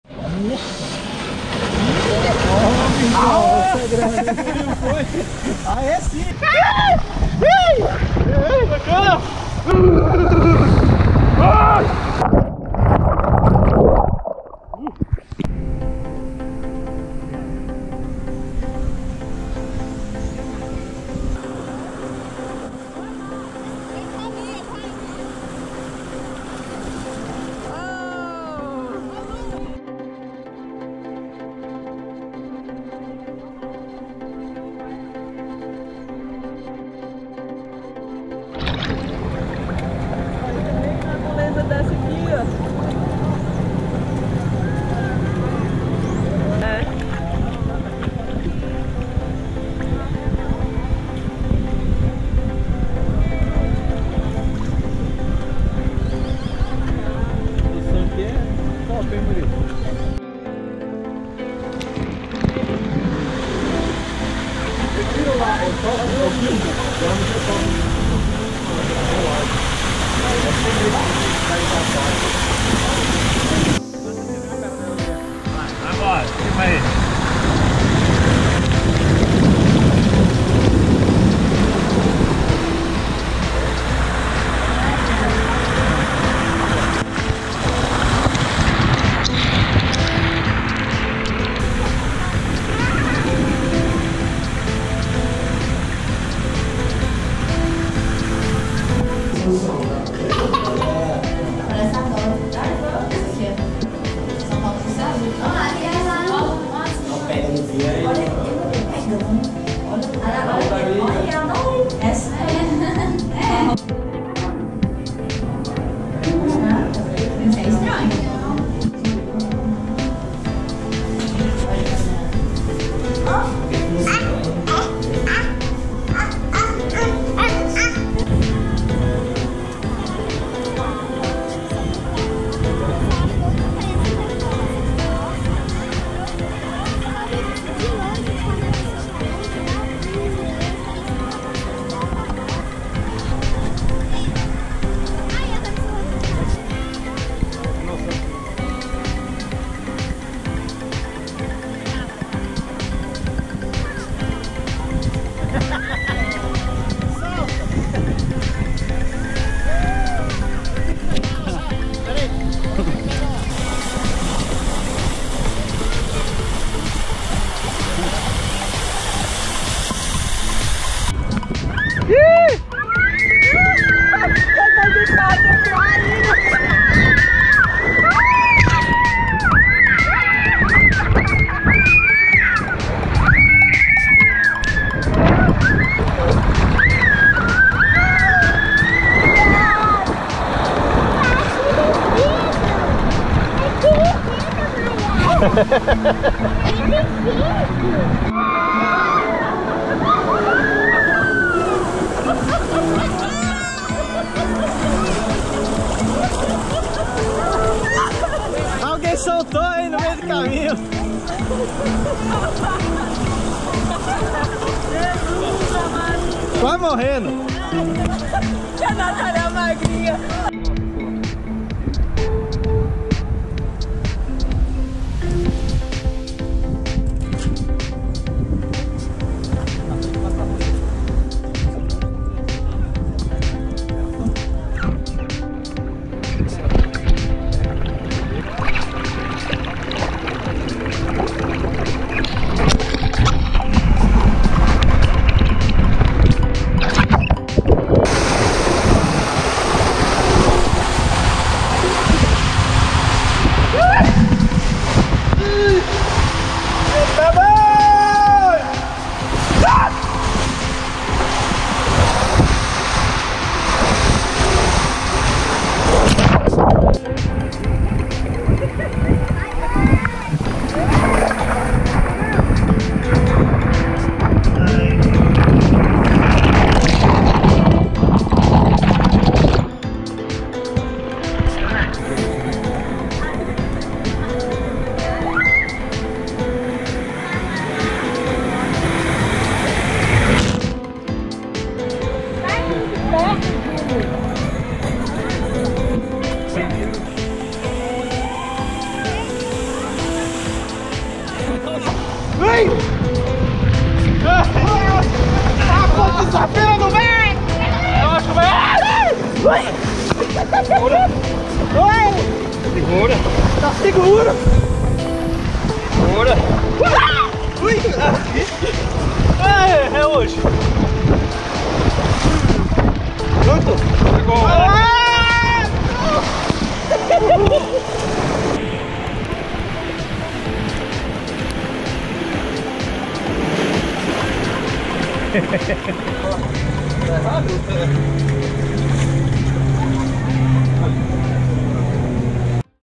Eco Parque - Arraial d'Ajuda - BA
O Arraial d’Ajuda Eco Parque é um parque aquático localizado no distrito de Arraial d'Ajuda na praia do Mucugê, em Porto Seguro, no estado brasileiro da Bahia.